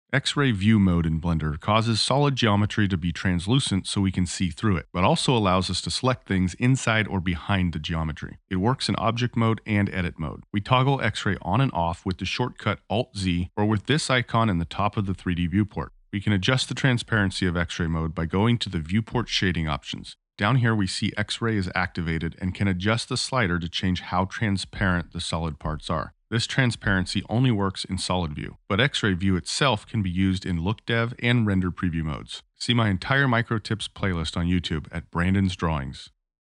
X Ray View Mode in Blender (Micro Tip)
In Blender we may need to view and select through objects or mesh. X-Ray view mode can be toggled on and off to allow selection through objects. Here's a quick rundown on how to use x-ray view in Blender in just 37 seconds!

Here's a blog post all about X-Ray View Mode in Blender